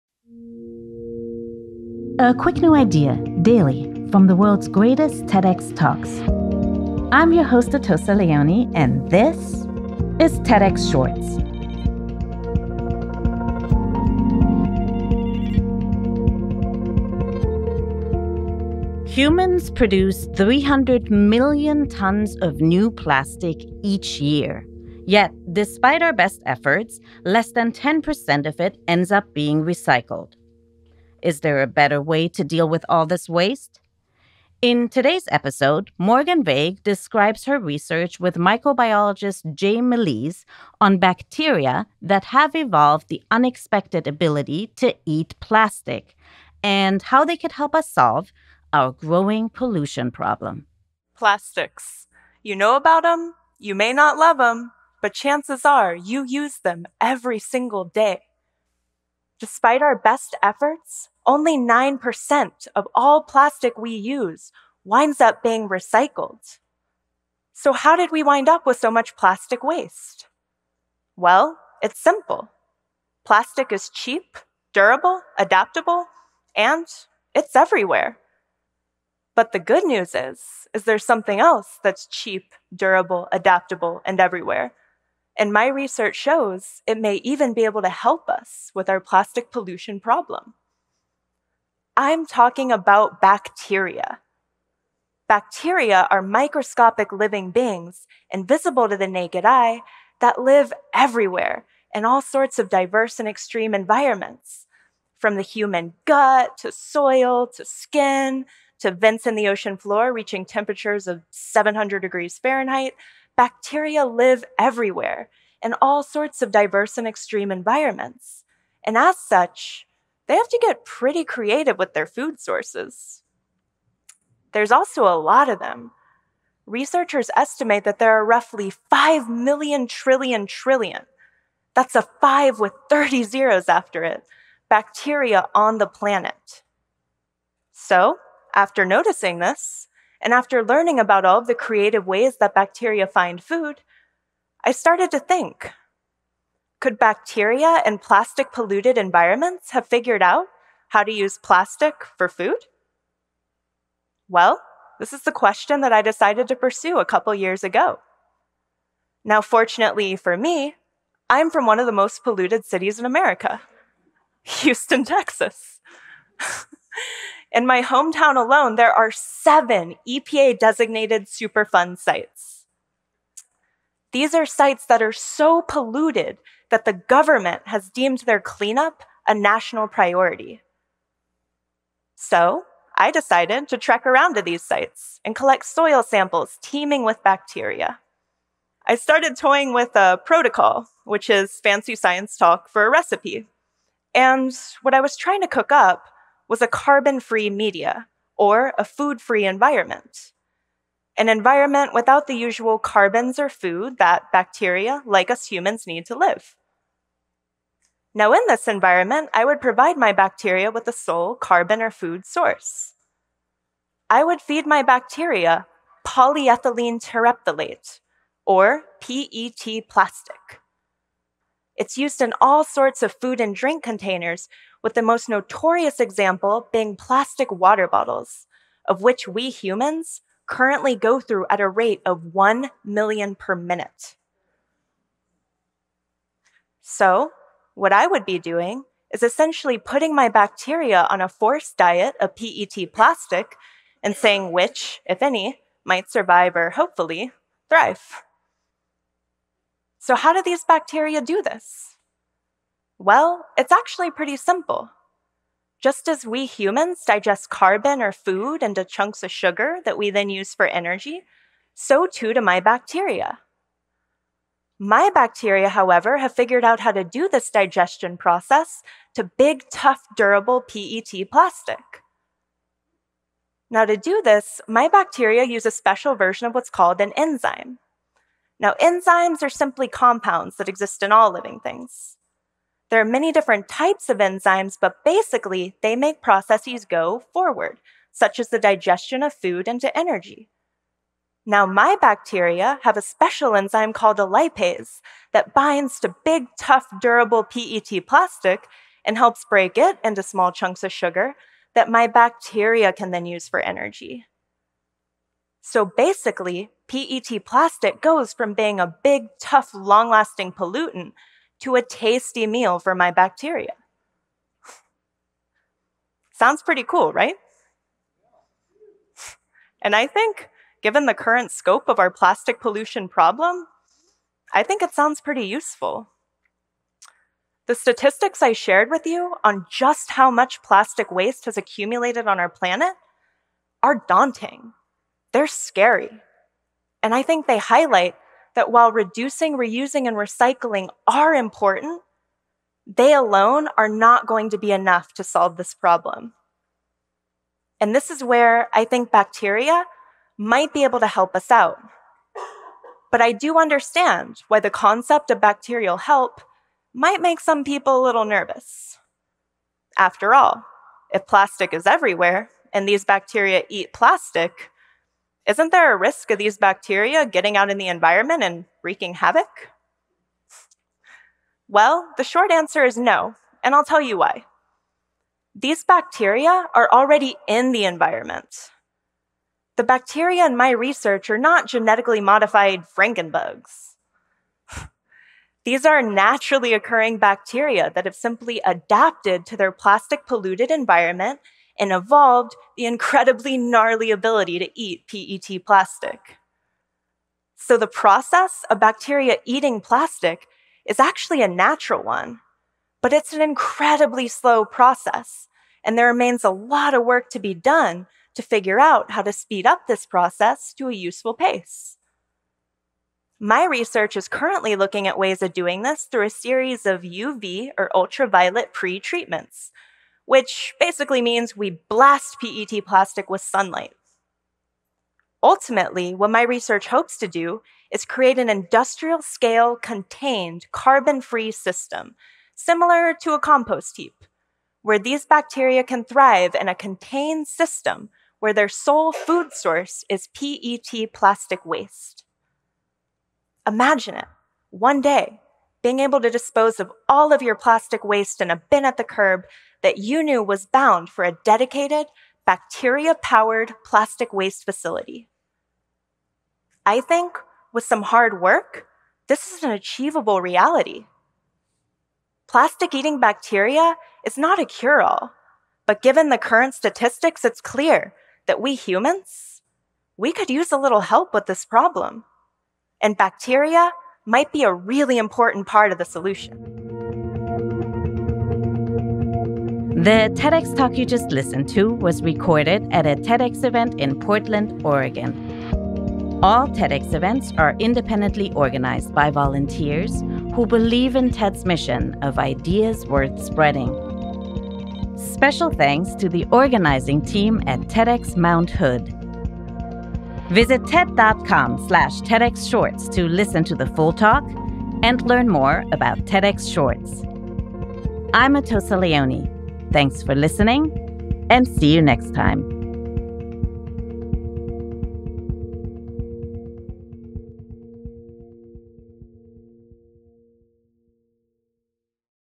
The bacterial solution to plastic pollution
Morgan Vague shares one way to solve our increasing pollution problem -- bacteria that has the ability to eat plastic.  This talk was filmed at TEDxMtHood.